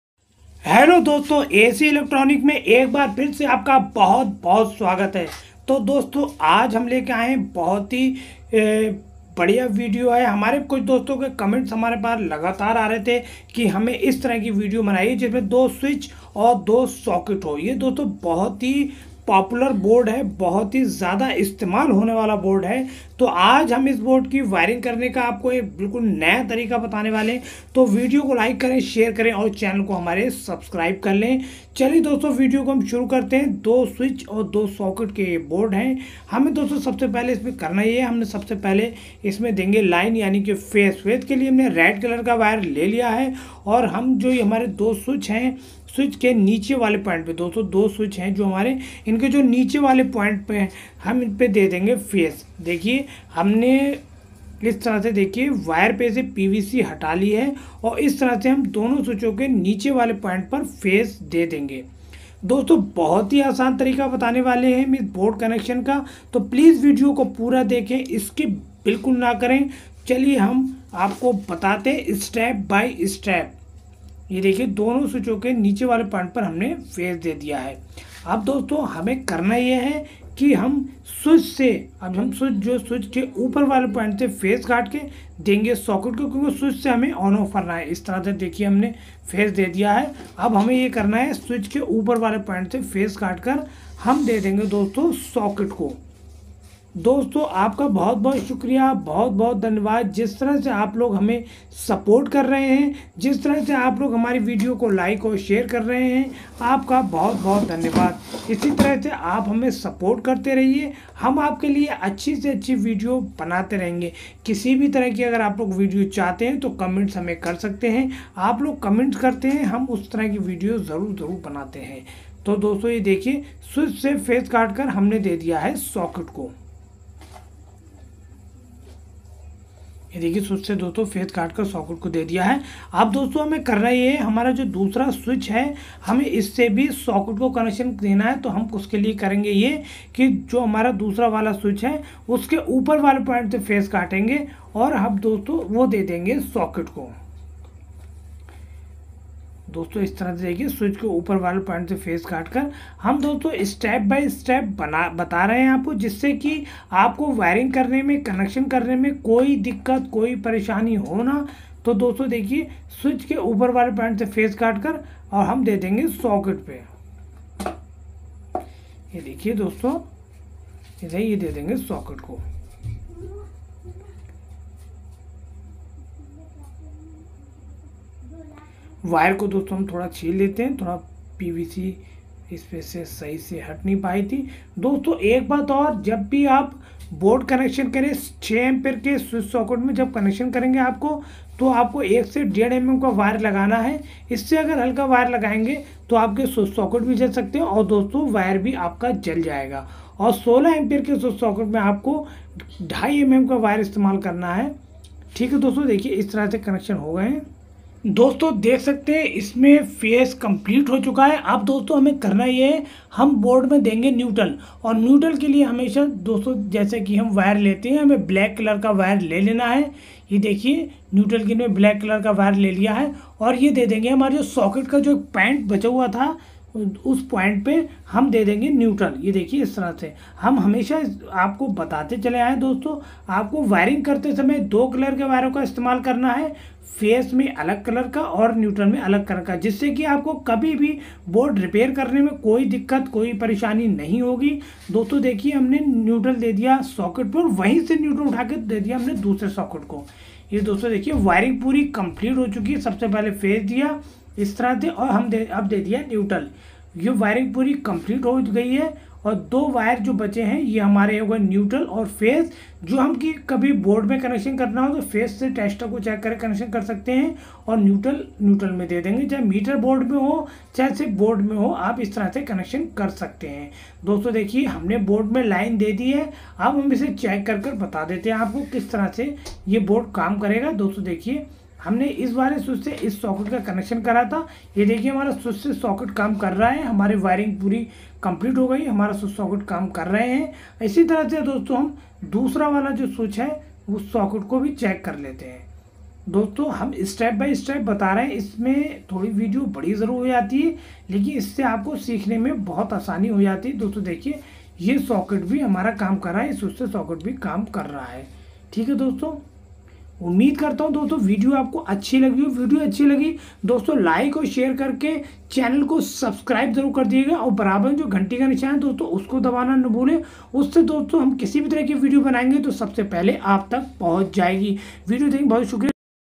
2 Switch, 2 Socket Board Connection With Fitting At Home ||How To Make Board Wiring
2 Switch, 2 Socket Board Connection With Fitting At Home ||How To Make Board Wiring Karen Ka Asaan Tarika